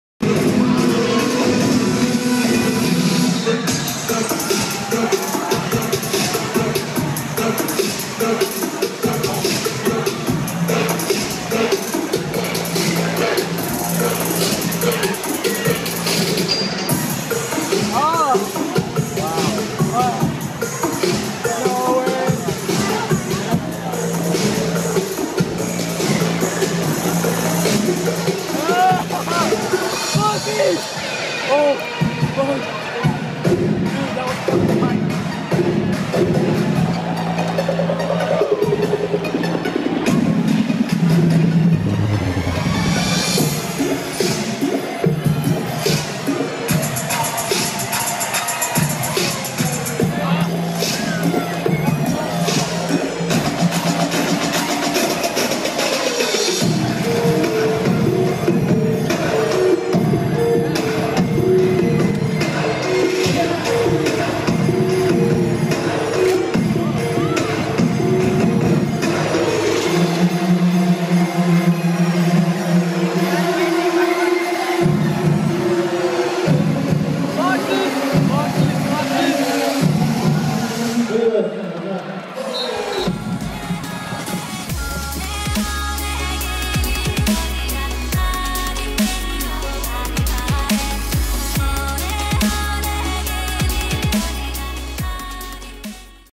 Sunrise 2013 - Ambience Double Light Show
Ambience Glovers: Euphoric & Daddy Long Fingers

Event: Sunrise 2013 in San Jose, CA